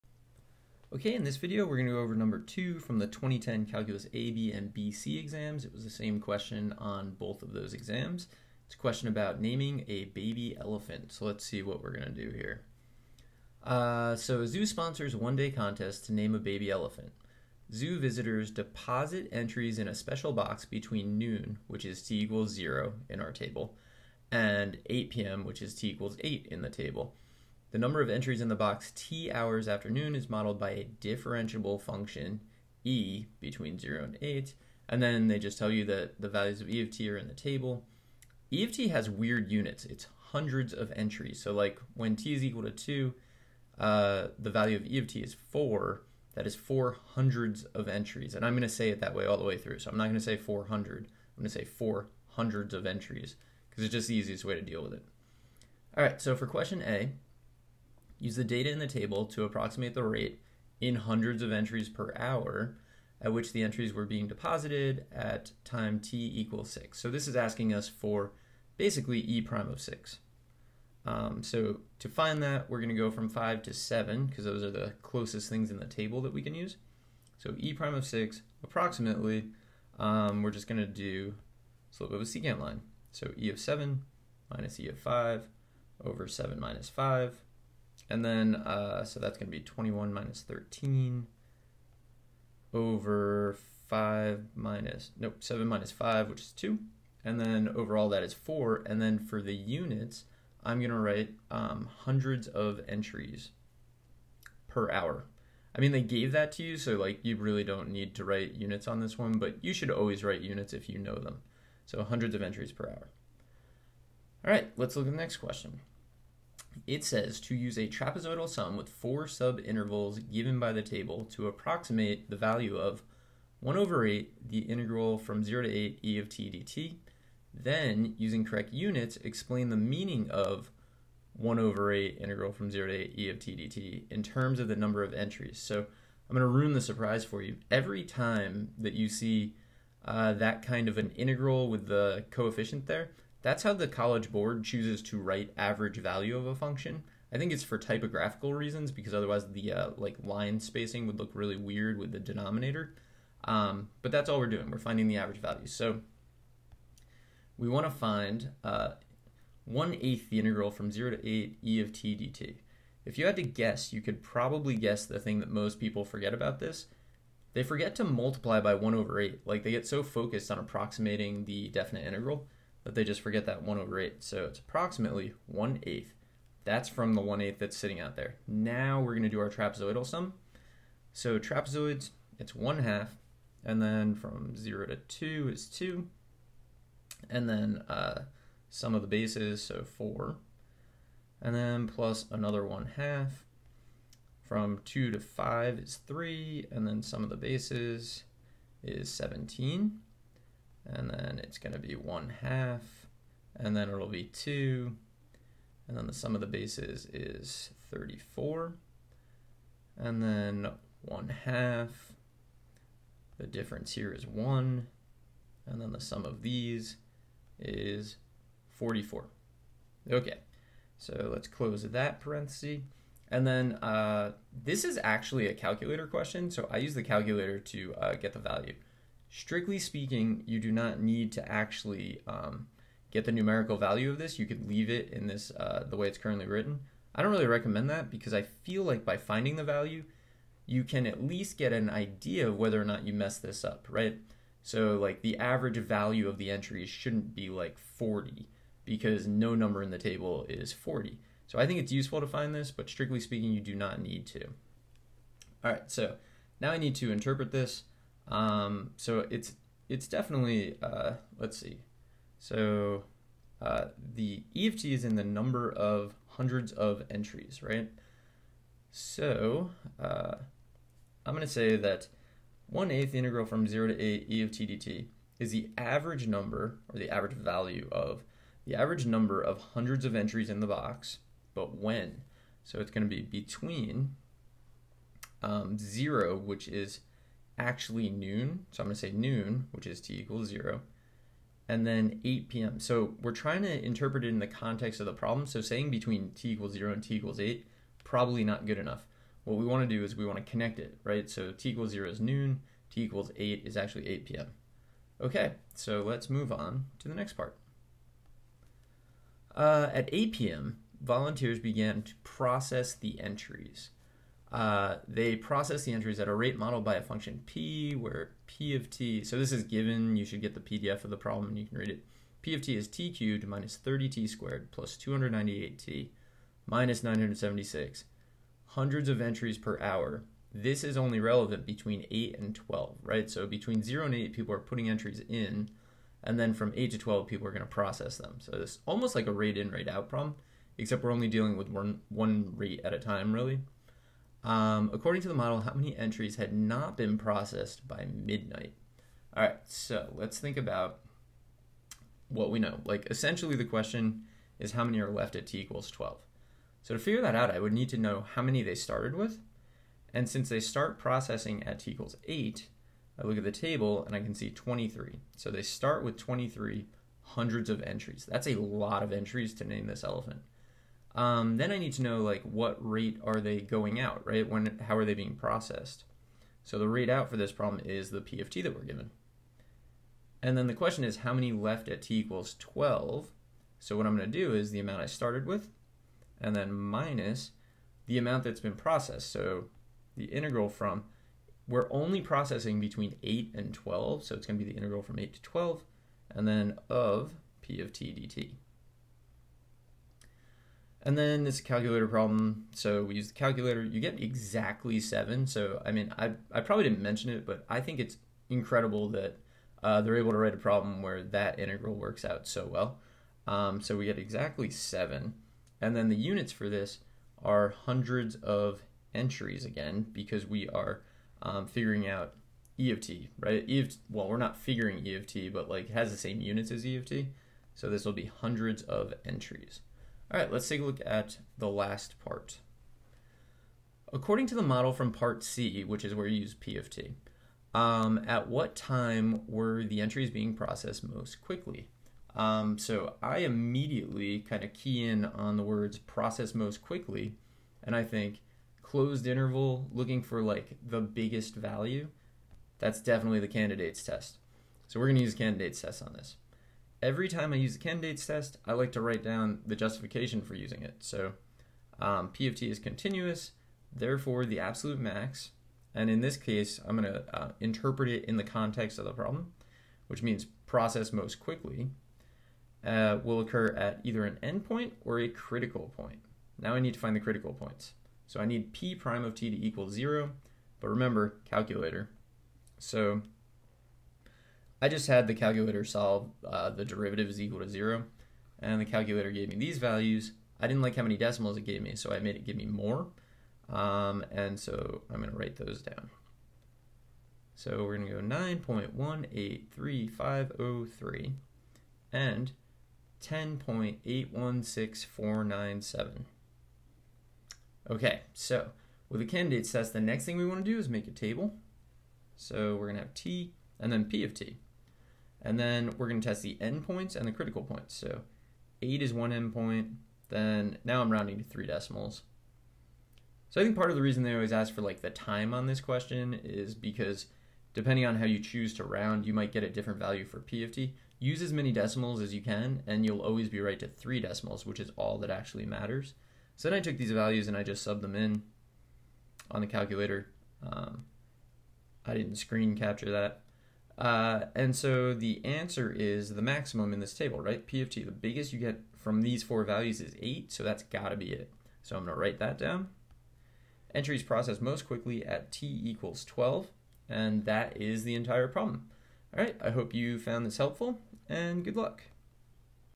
2010 AP Calculus AB & Calculus BC Exam FRQ #2
2010 AP Calculus AB and AP Calculus BC Exam Free Response Question #2

Table problem!

Topics: Approximate instantaneous rate of change (derivative) with secant line; average value of a function; interpreting average value; trapezoid rule; rate in, rate out; Candidates test for absolute maximum